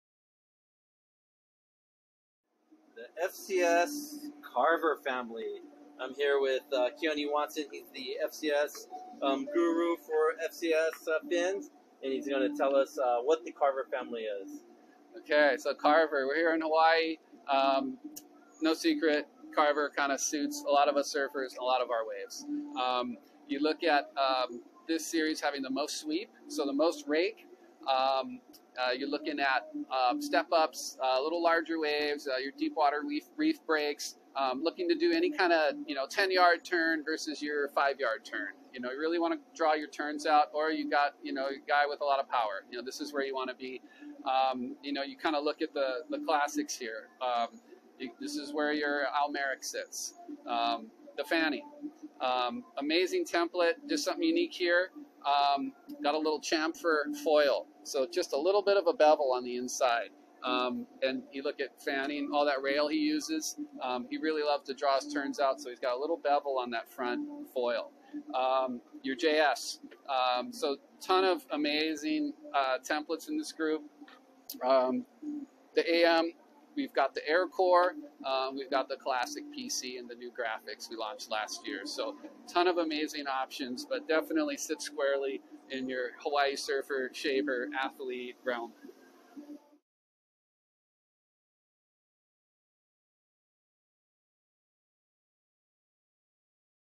Learn More About The FCS II CARVER Family | Hawaiian South Shore
Fins with an extensive rake, such as those in the Carver family, will turn in a longer, more drawn-out arc and provide more stability and control. This makes them conducive for point breaks and other open-faced waves, typically on boards with a bit more rocker.

The Carver fins are for power.

Your  Surf Authority in Honolulu Hawaii